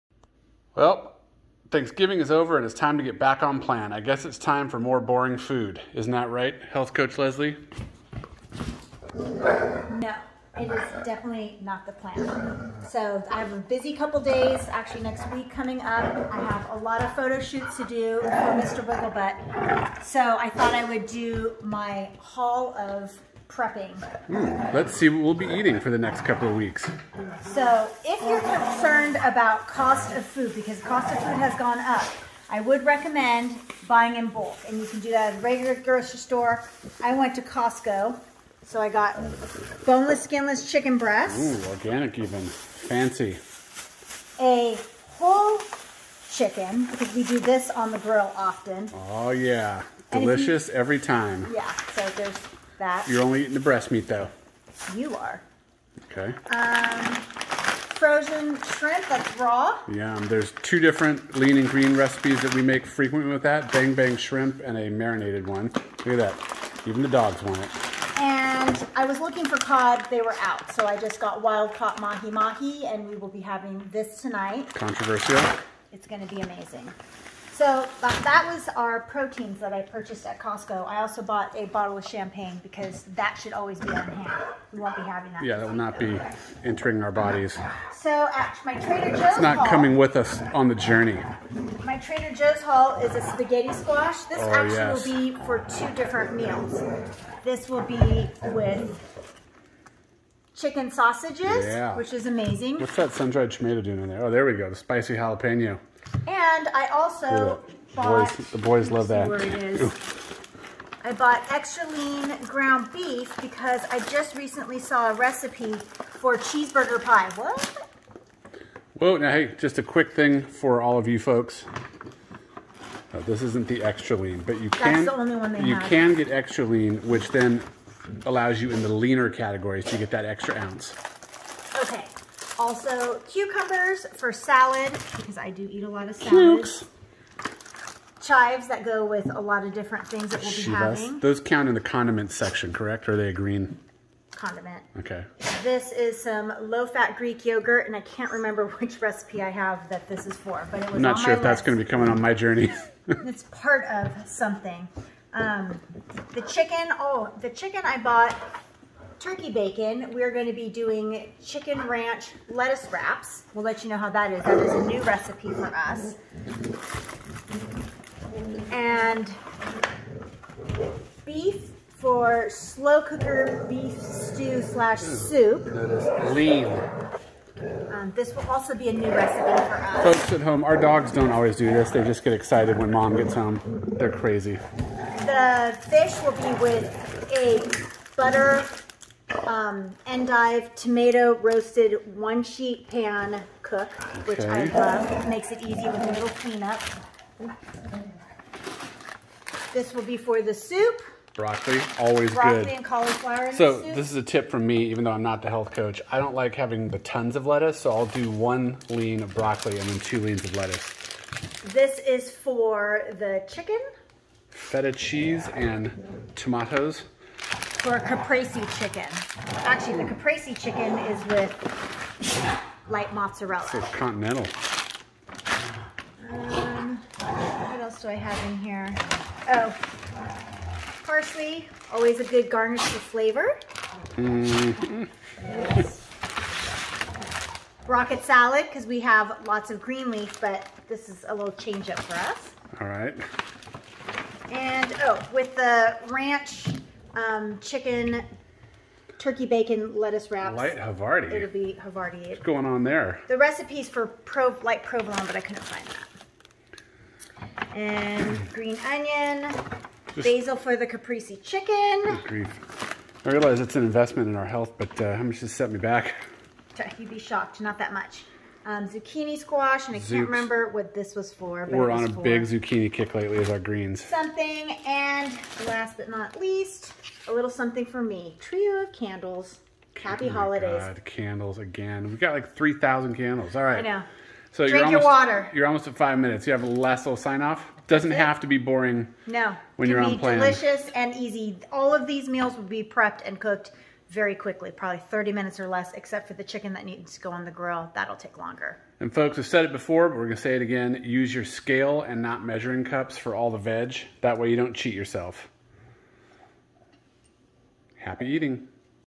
Healthy Shopping Tips From Leslie
The holidays don't need to mean boring food if you're trying to stay on track.  Here's Health Coach Leslie's post Thanksgiving shopping haul of healthy foods for great meals!